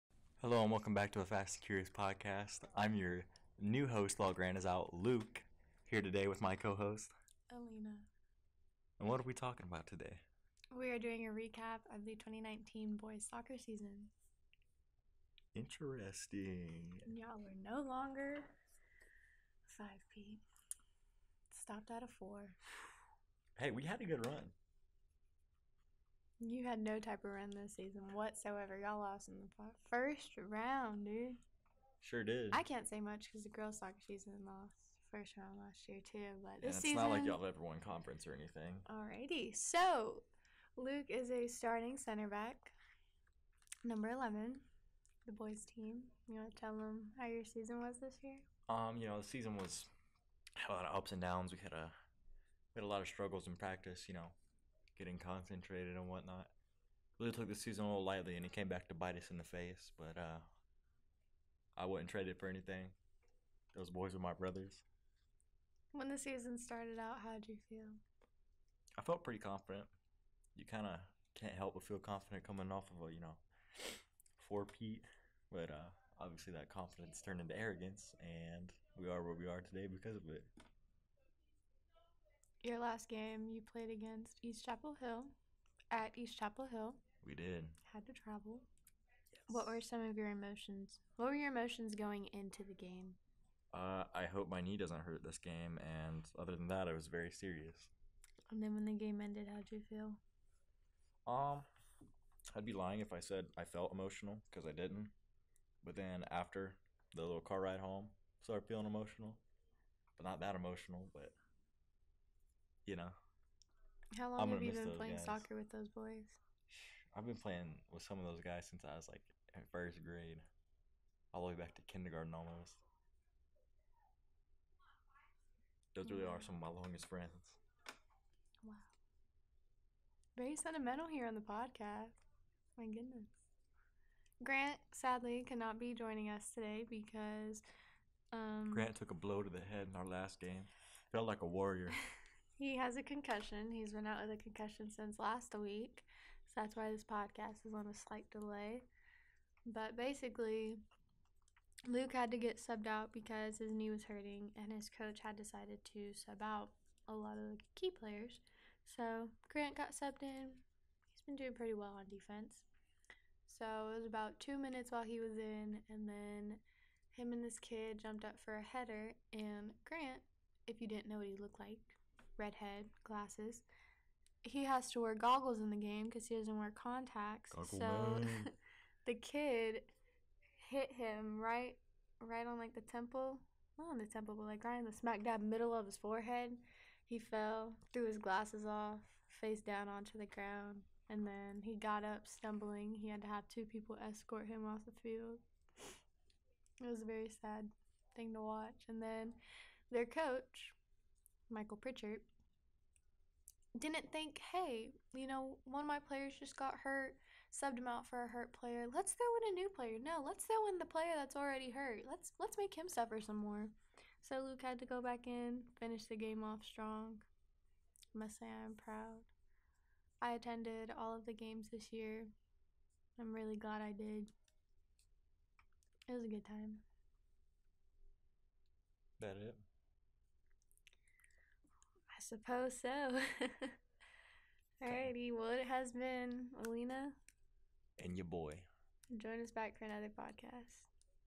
Fast & Curious Podcast - 2019 Soccer Recap
In this podcast grant is concussed so Luke and Alina take over the podcast game and talk over the 2019 boys soccer season.